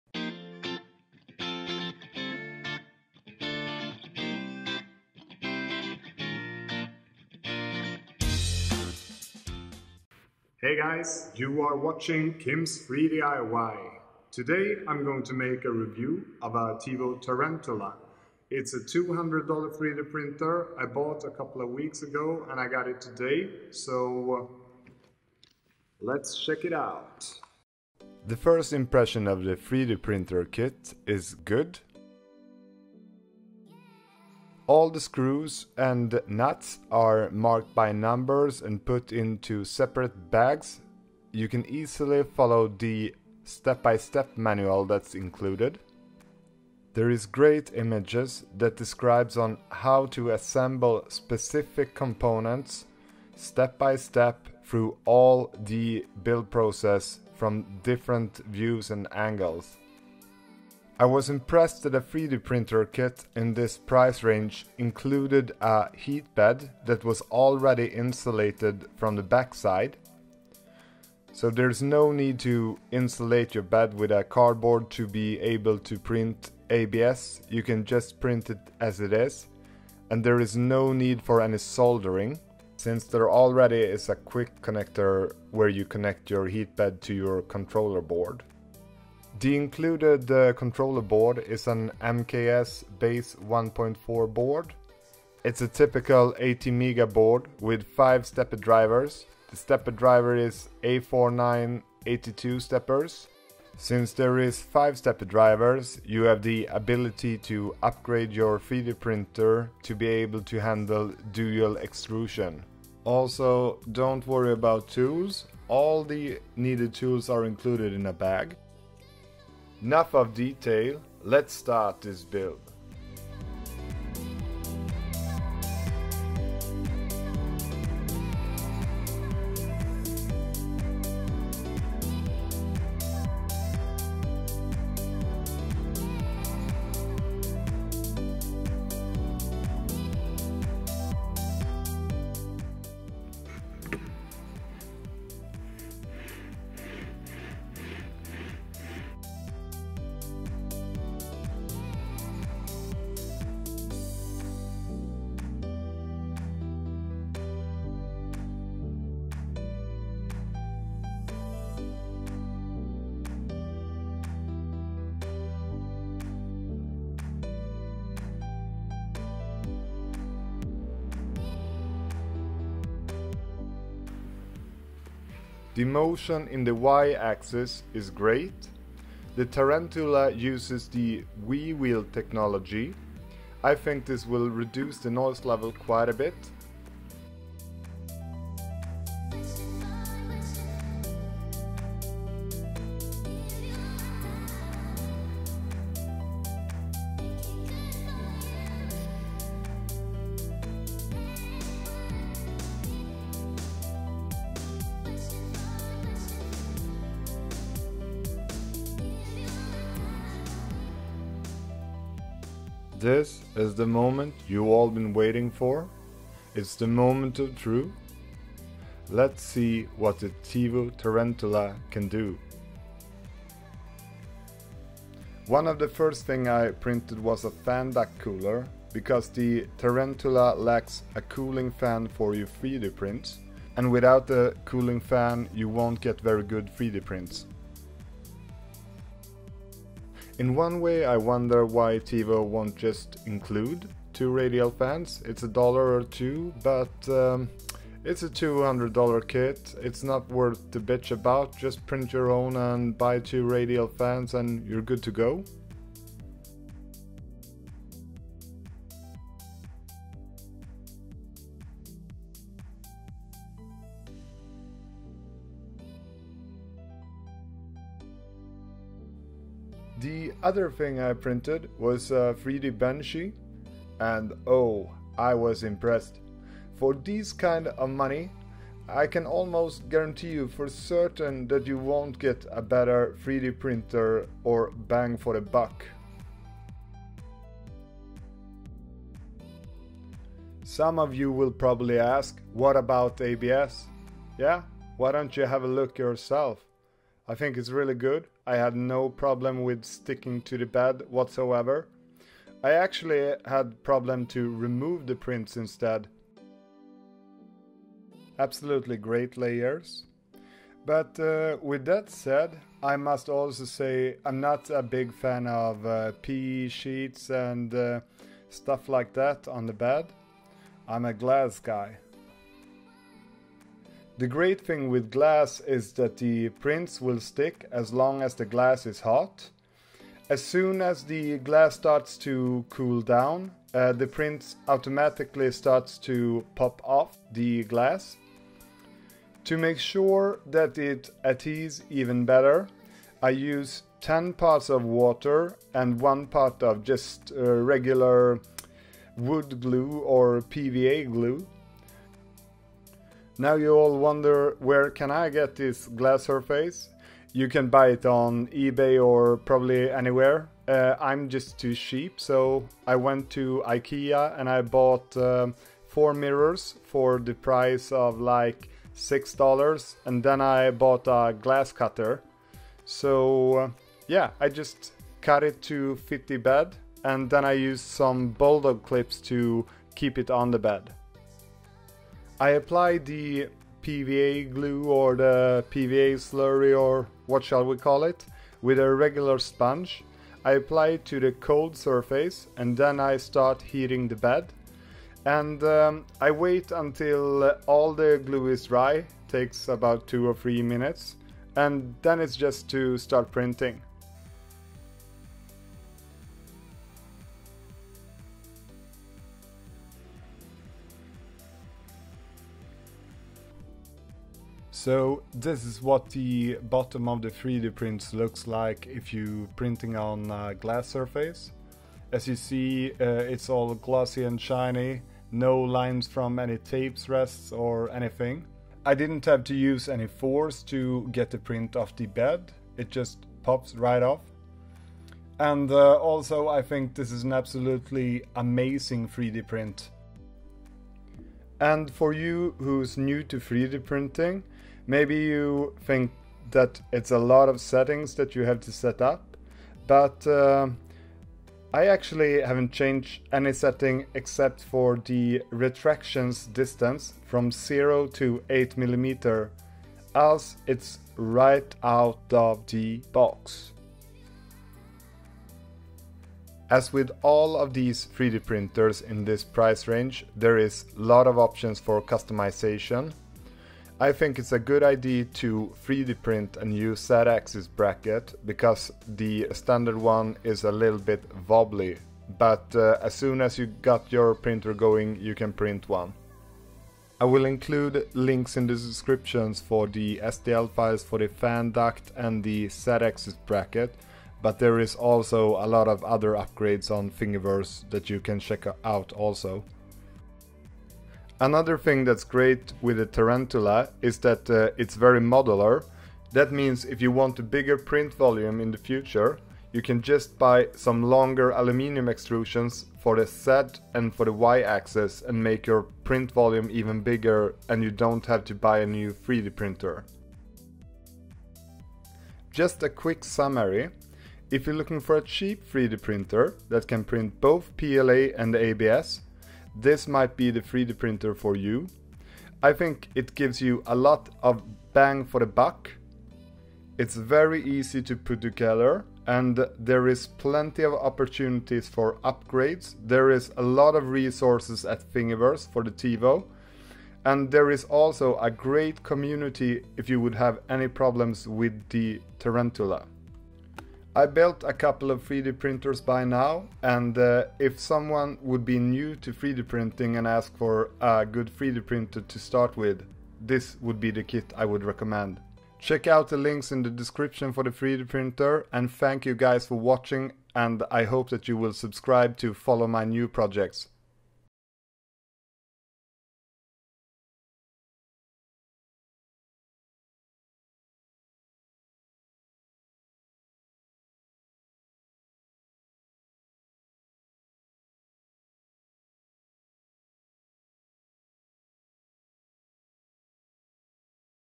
Tevo Tarantula the best cheap 3D-printer? Review
Its an absolutley amazing 3d-printer for its price!

I also recommend the CR-10 if you want bigger print volume